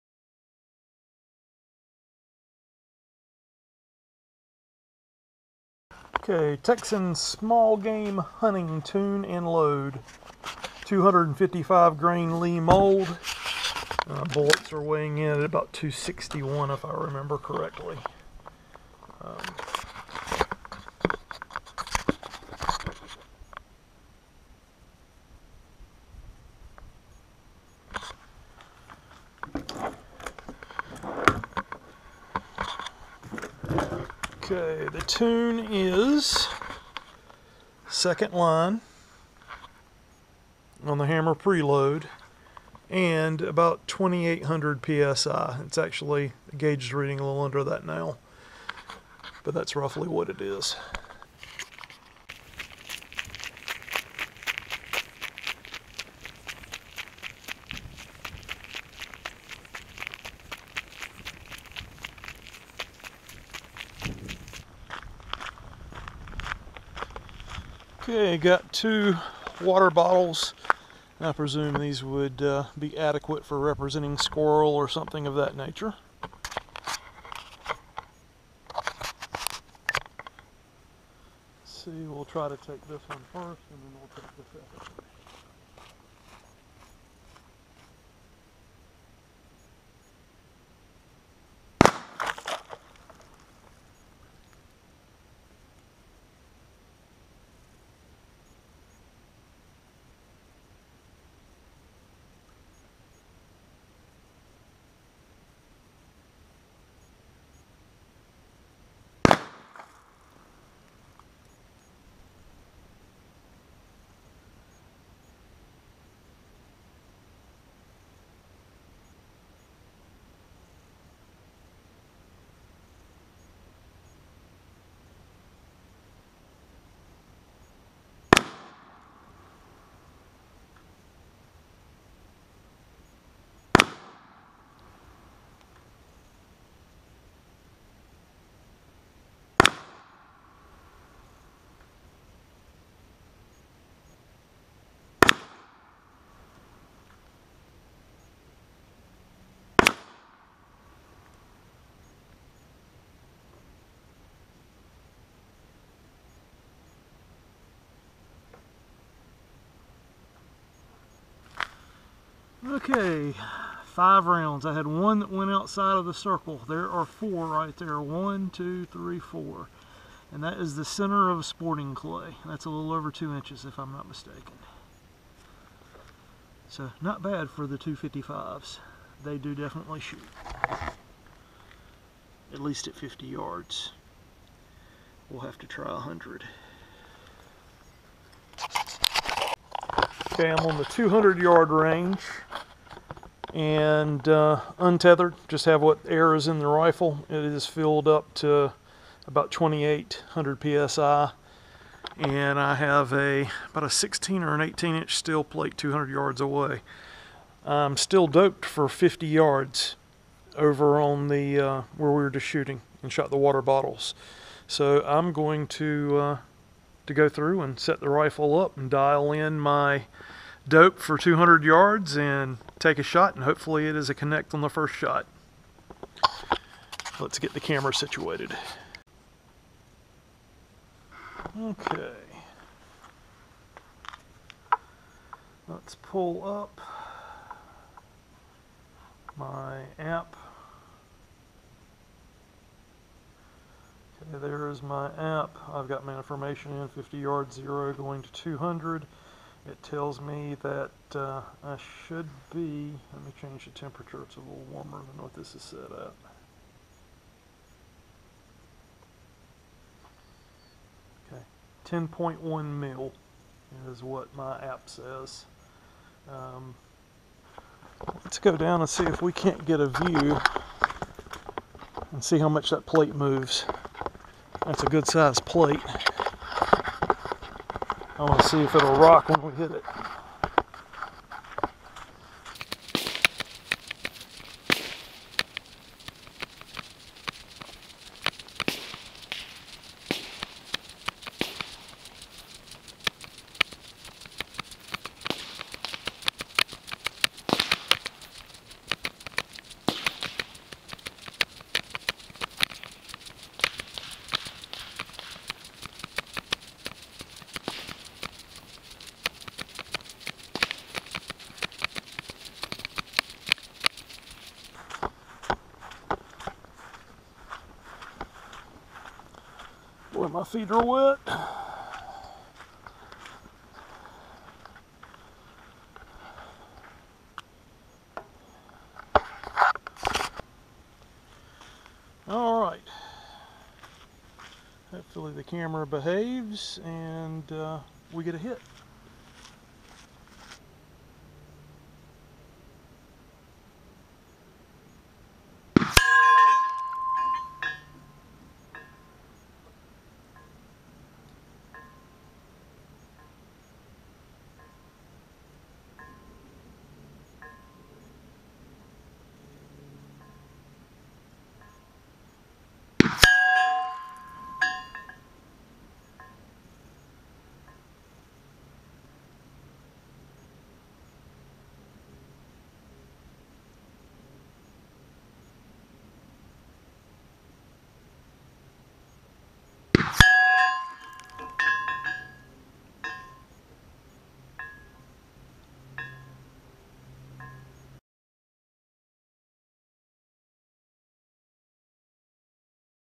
.457 Texan CF - Lee 255-.452 Powder Coated to .457 - 50 & 200 Yds
Making the Lee 255 grn - .452 Mold work pretty well in the Texan .457 by double powder coating and sizing to .457